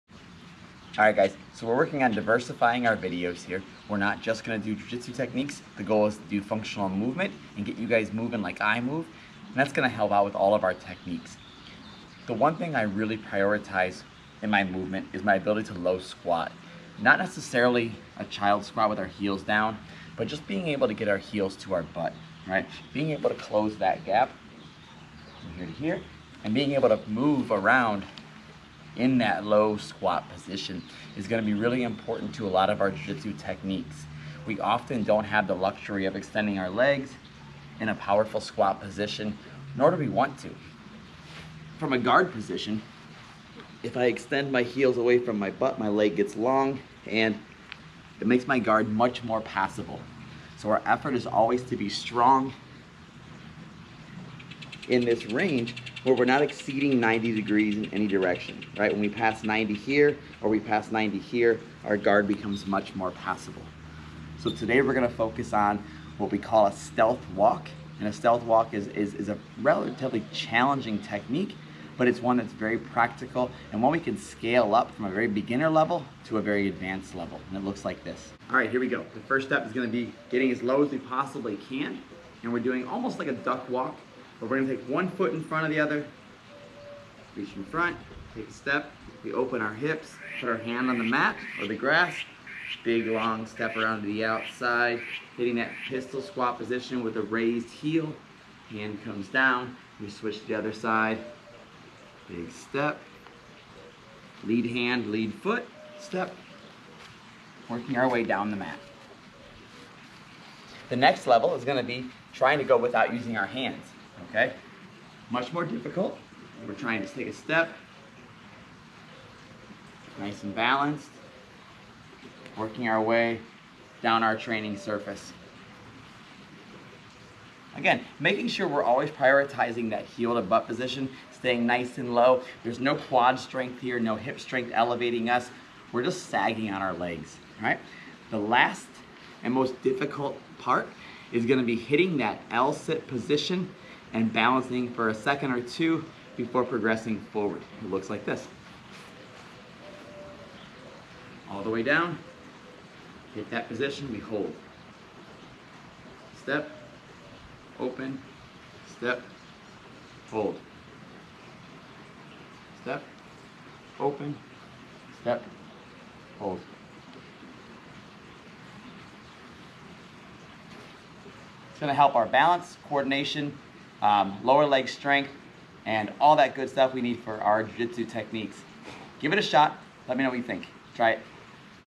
The Stealth Walk my favorite functional movement drill for bjj
This is something I do literally every day. The Stealth Walk focuses on our balance, coordation and lower leg strength and is a great warm up drill for Jiu-jitsu. Give it a shot!